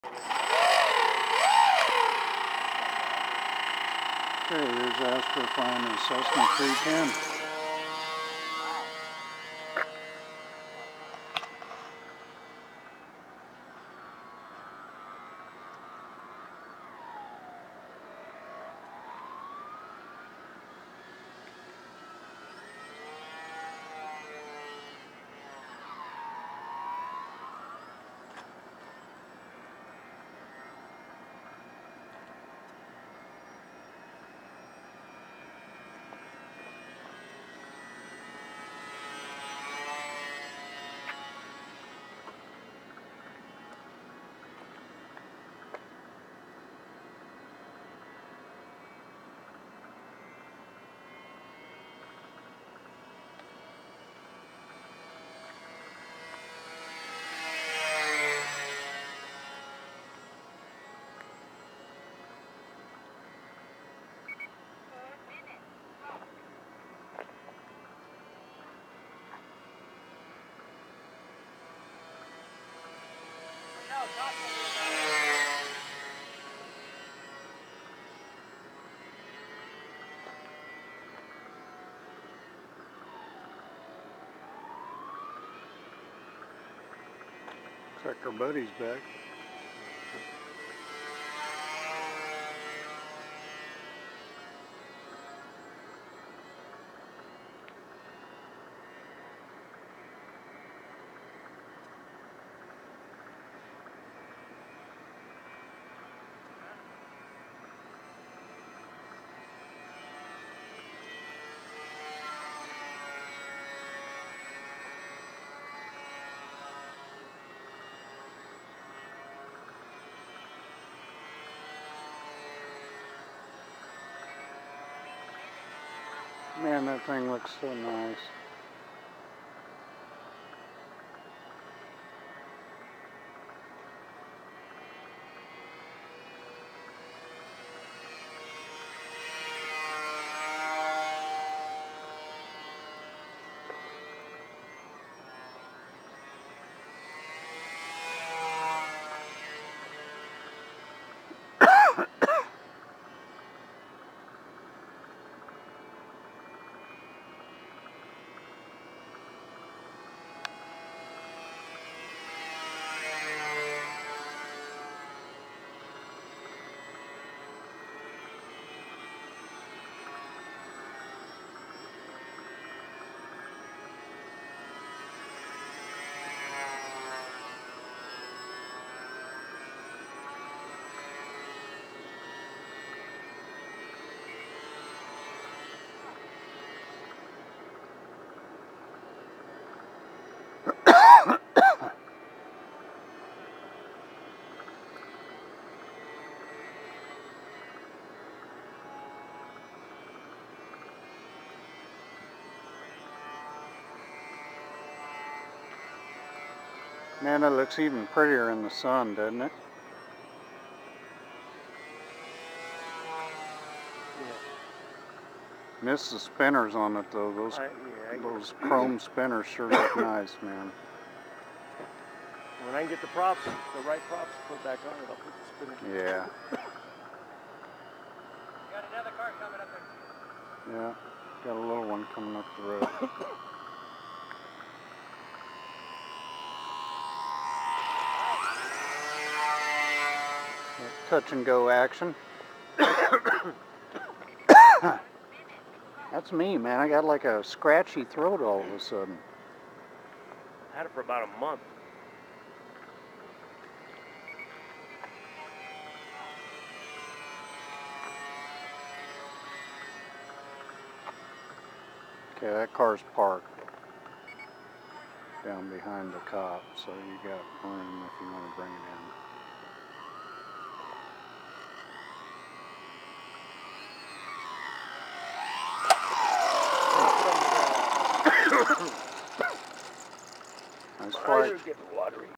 Astras Cessna 310 Flight 12-12-10
Here is Astra flying his Cessna 310 and smooth as always.

"Raw Footage No Edit"